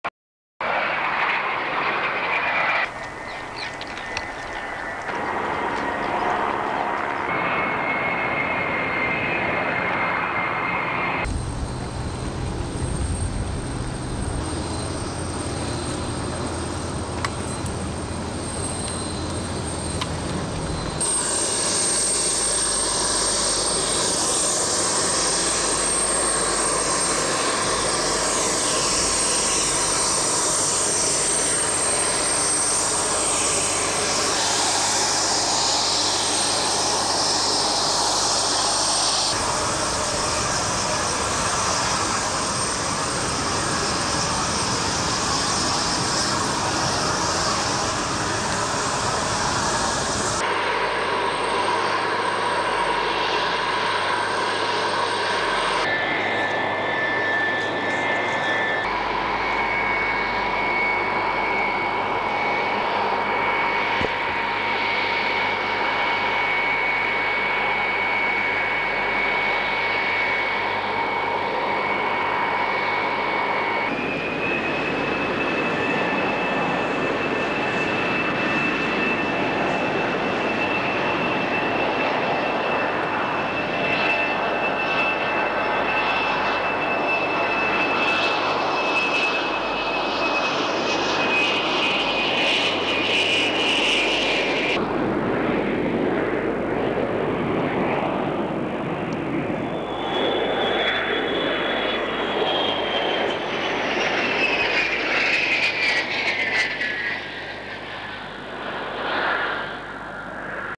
Amphibious Aircraft Jet
Beriev BE-42  A-40 Albatros Mermaid Landing and take off on Waitemata harbour auckland new zealand